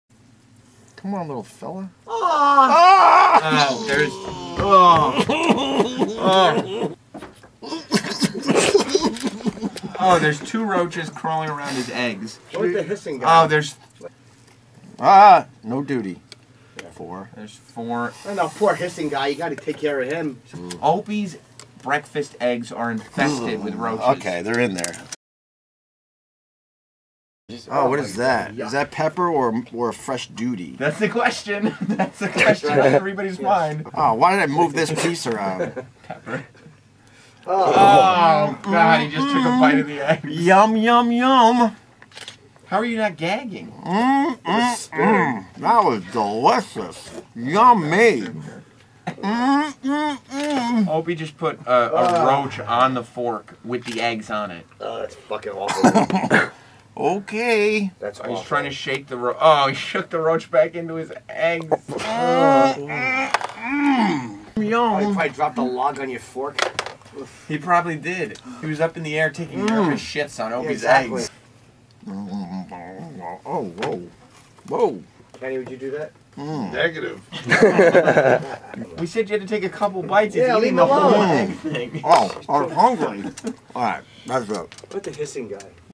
Cockroach Breakfast
We were discussing cockroaches in your food. Jim Norton said he had cockroaches crawl across his food twice in nice nyc restaurants. One time he said that a cockroach crawled from underneath his fries. Of course, he stopped eating and left. I said, I wouldn't be happy about it but it's not that big of a deal. I told the guys to bring in cockroaches and I would let them crawl around my breakfast as I ate. Honestly, I didn't think they would actually get the cockroaches BUT they did. At that point I wasn't going to back down from what I said. So, here you go...a quick video showing that it really isn't a big deal.